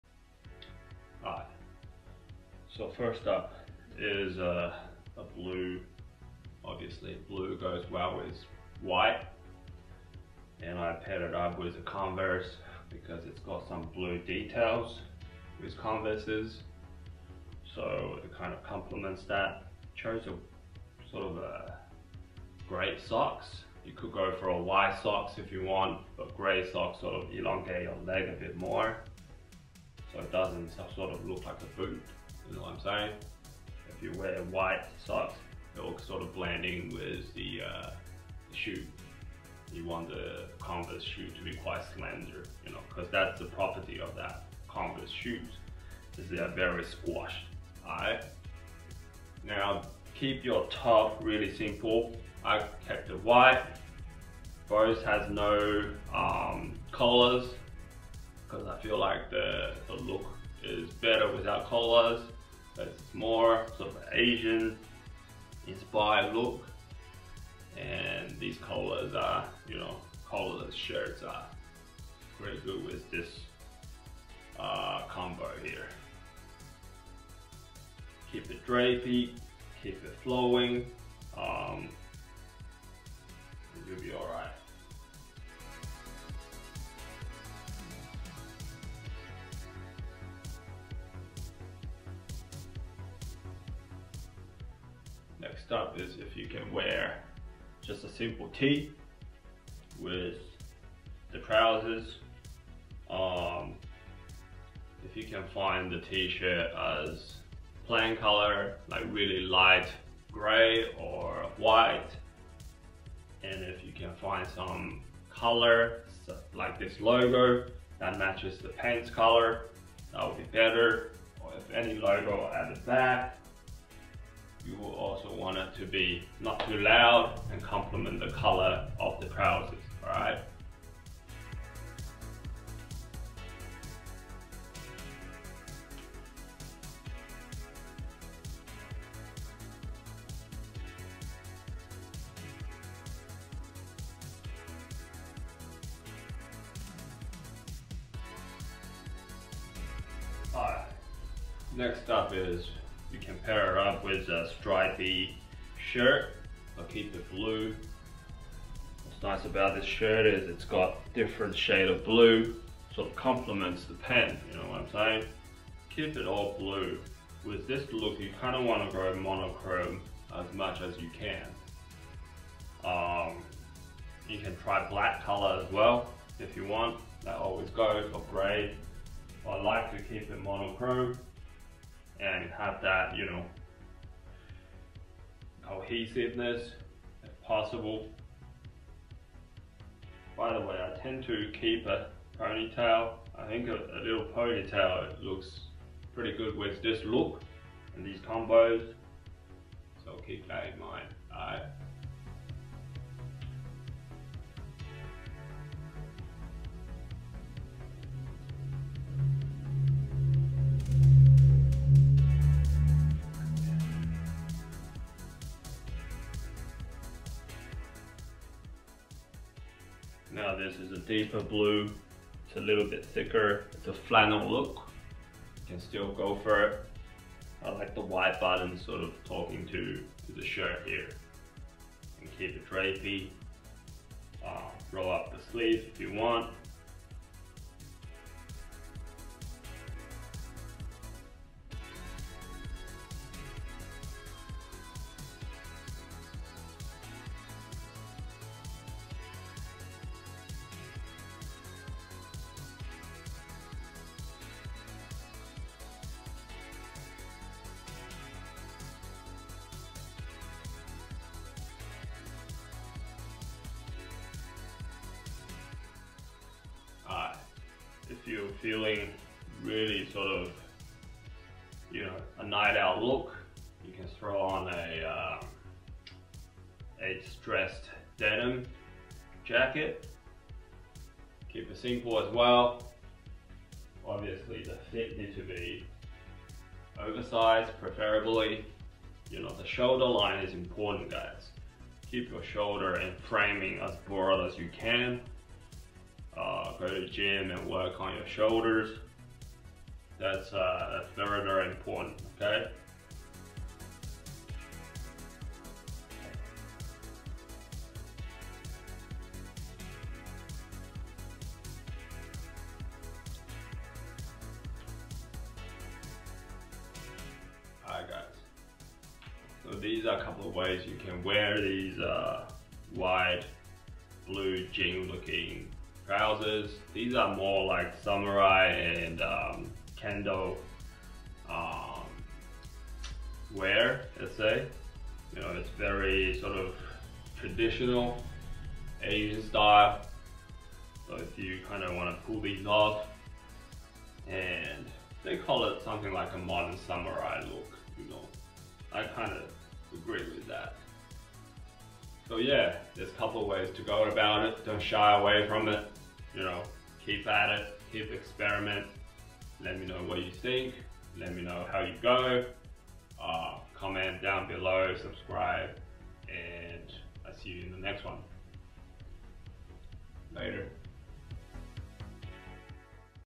How to wear a wide blue pants (modern warrior _ samurai style) [Vertical video]
this is how I go out normally, not a cosplay.